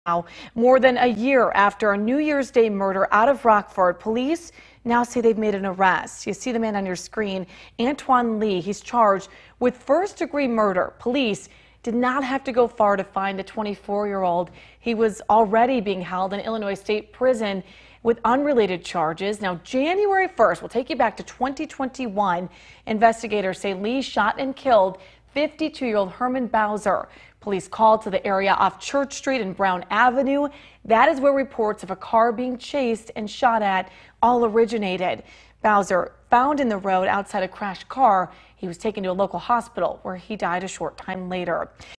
Rockford man charged with murder for 2021 New Years Day killing
Antwone Lee, 25, has been charged with the murder of 52-year-old Herman Bowser, who was shot to death during a vehicle chase in January 2021.